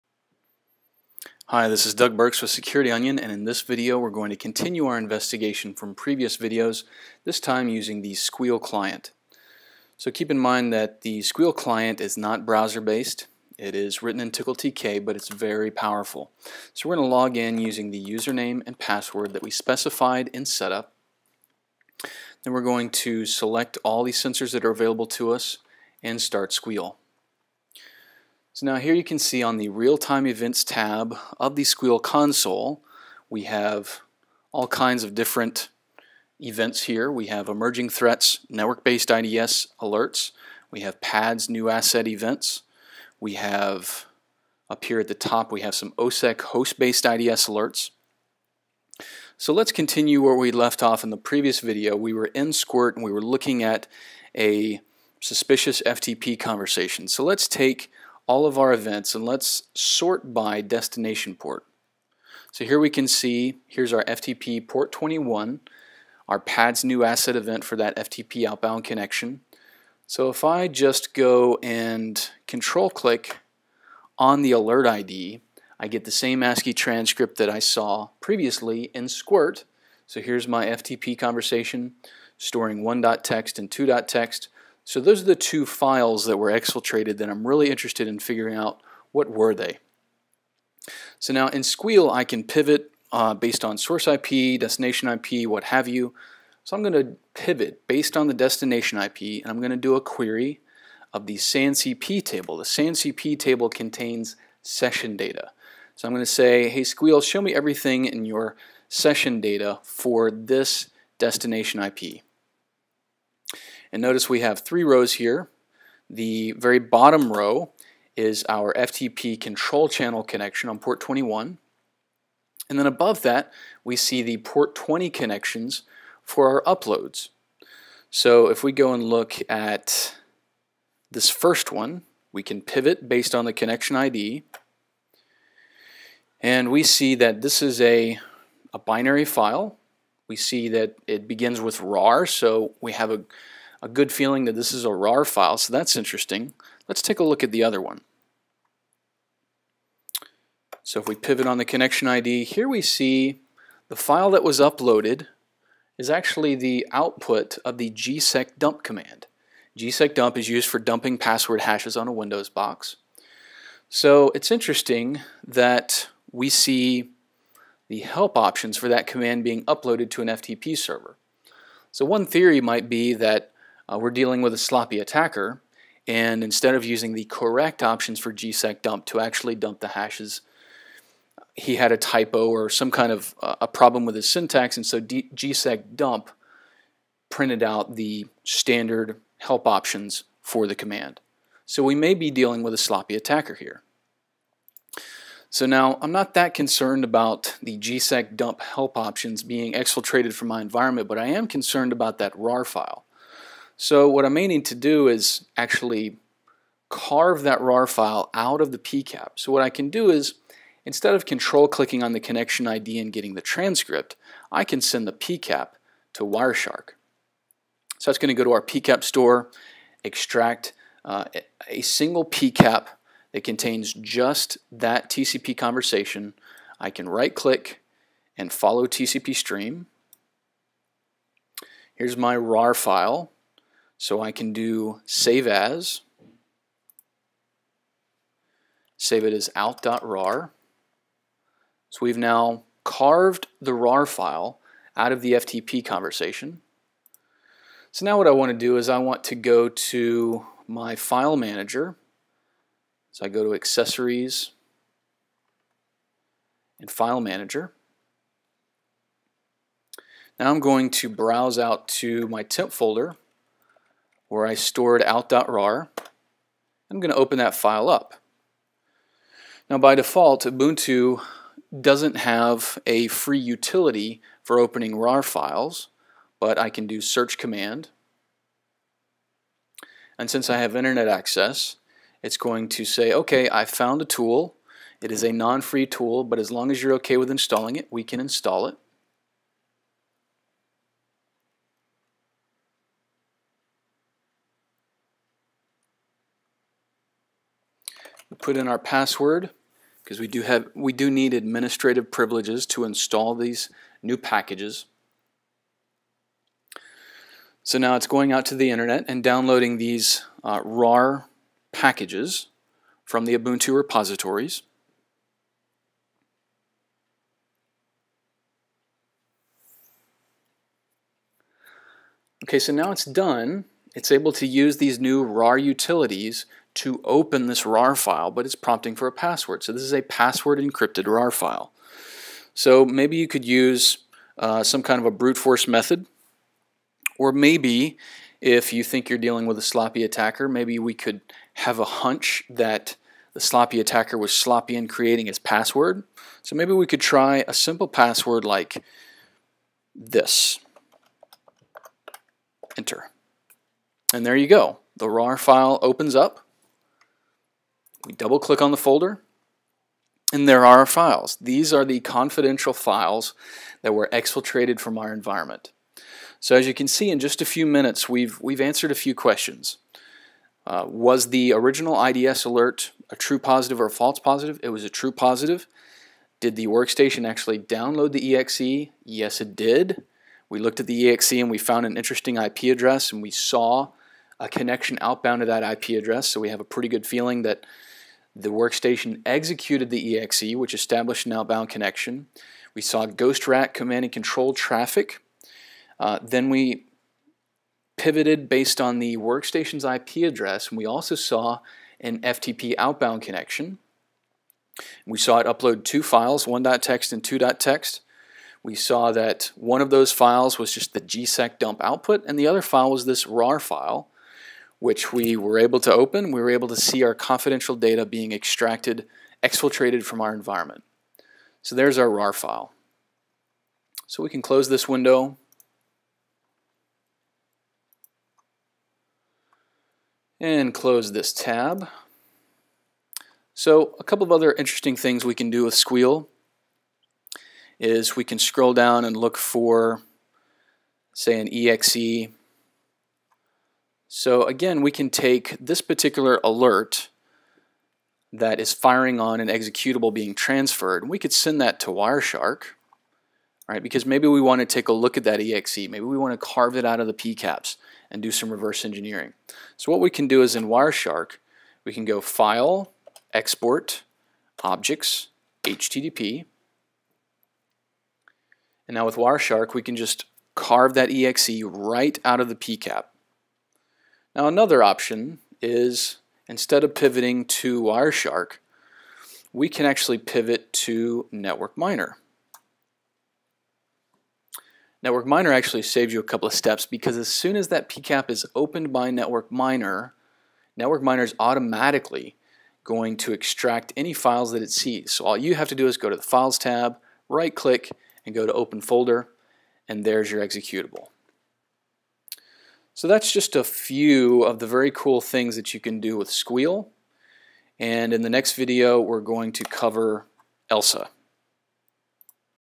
Security Onion and Sguil
In this video, we use Sguil to continue our investigation.  We pivot to Wireshark and extract a RAR file that was exfiltrated from our environment.  We then open the RAR file to see our confidential data that now belongs to our adversary.  If you have questions or problems, please use our mailing list: